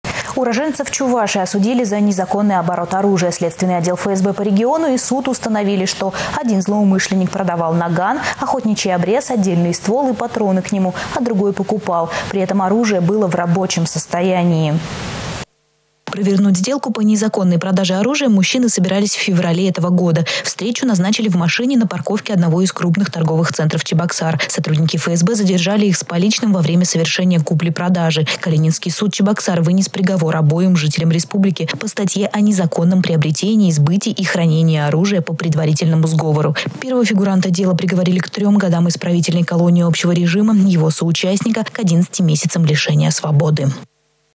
Уроженцев Чувашии осудили за незаконный оборот оружия.
Уроженцев Чувашии осудили за незаконный оборот оружия. Следственный отдел ФСБ по региону и суд установили, что один злоумышленник продавал  «Наган», охотничий обрез, отдельный ствол и патроны к нему, а другой покупал. При этом оружие было в рабочем состоянии